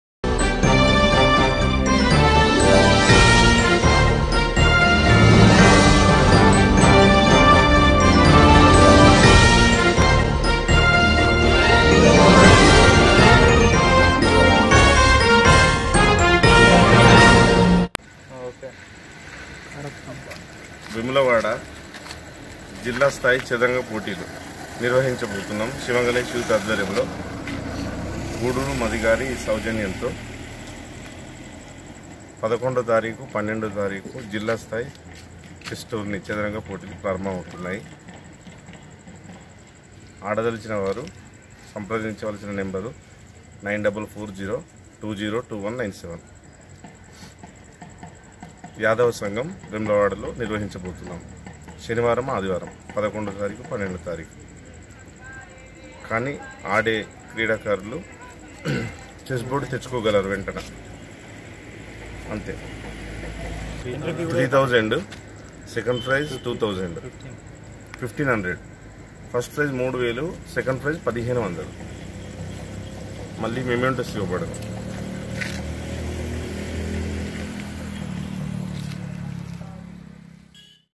వేములవాడలో డివిజన్‌ చదరంగ పోటీలు..
వేములవాడలో డివిజన్‌ చదరంగ పోటీలు..                                                                                         వేములవాడ పట్టణంలో శివగణేష్ యువజన సంఘం ఆధ్వర్యంలో  సిరిసిల్లా డివిజన్‌ పోటీలను నిర్వహిస్తున్నట్లు నిర్వాహకుడు దెబ్బటి నవీన్‌ ఒక ప్రకటనలో తెలిపారు. ఆసక్తి ఉన్న అభ్యర్థలు సంప్రదించాలని కోరారు.  11, 12వ తేదిల్లో పోటీలు నిర్వహిస్తున్నట్లుగా పేర్కొన్నారు. పోటీల్లో పాల్గొనే  ఆటగాళ్లు 10వ తేదిలోపు నిర్వాహకులను సంప్రదించాలని కోరారు.
ప్రథమ బహుమతి రూ.3వేలు, ద్వితియ బహుమతి రూ.1500 అందజేయనున్నారు.
వేదిక: యాదవ కల్యాణ మండపం, వేములవాడ
సంప్రదించాల్సిన ఫోన్‌ నం.9440202197
రాజన్న సిరిసిల్ల స్టాఫ్ రిపోర్టర్.                                               డి ప్రవీణ్.                                                                                 వేములవాడ